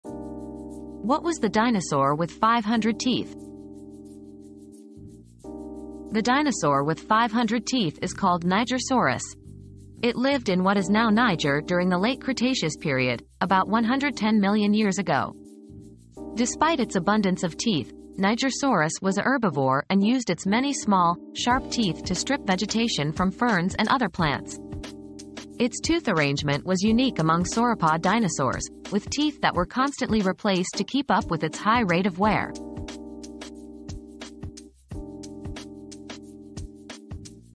What was the dinosaur with 500 teeth?
Discover the Dinosaur with 500 Teeth: Meet Nigersaurus! This fascinating herbivore from the Late Cretaceous period had an impressive set of teeth, but don't let that fool you - Nigersaurus was a gentle giant who used its unique tooth arrangement to munch on ferns and other plants. Join us as we explore the incredible world of Nigersaurus and uncover the secrets of this ancient beast's toothy grin!